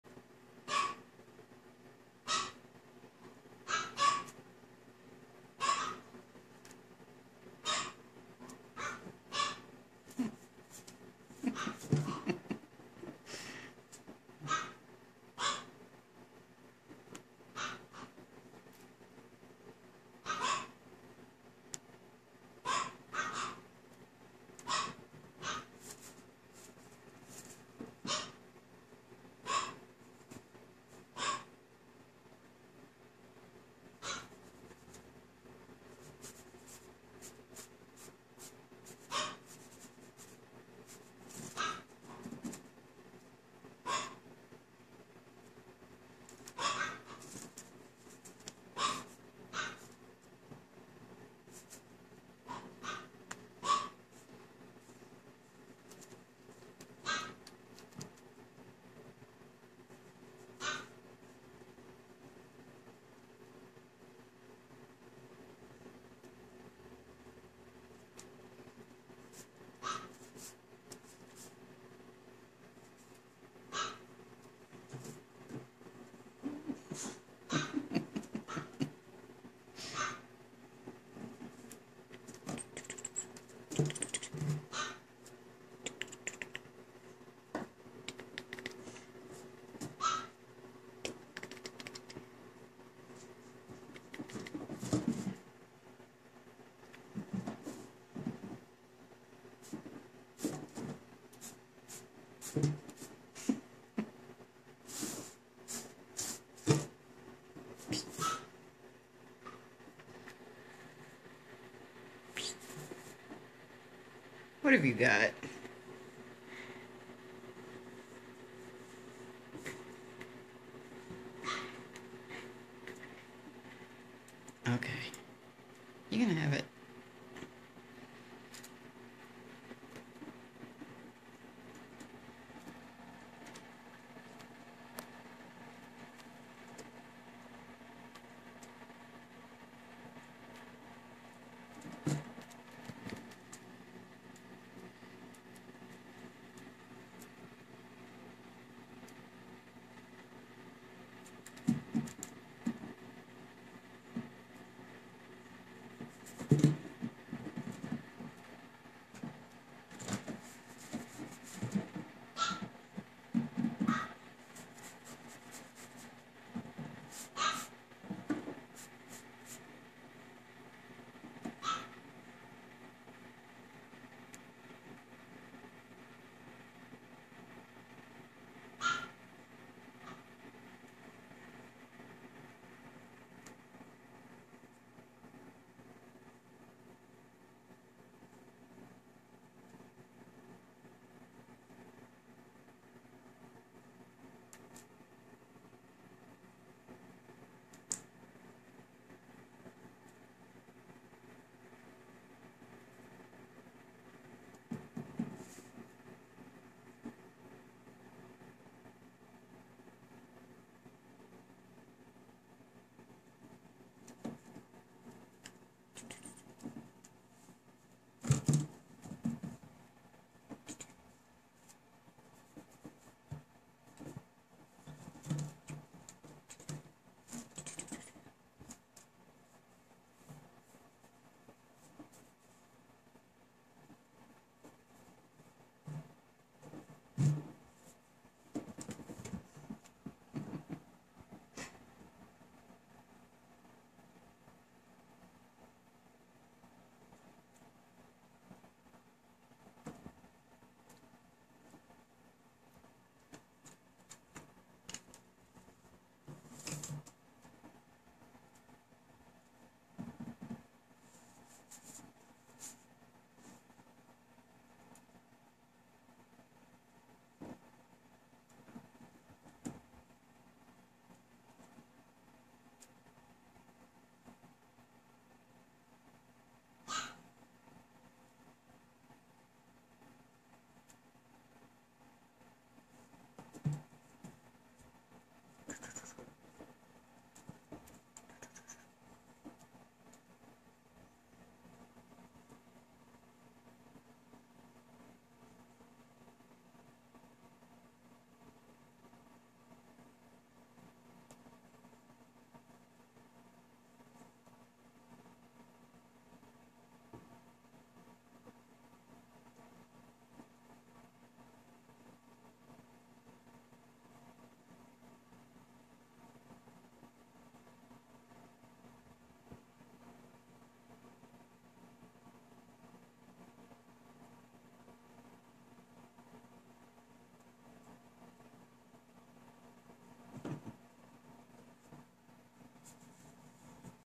Baby Squirrel Norman Playing
Little Norman is growing up so fast!
He absolutely loves playtime. He is learning to climb trees and forage.
Just a few more milestones and he will be ready to move to an outside enclosure.
Once step closer to a life of freedom that all wild animals deserve!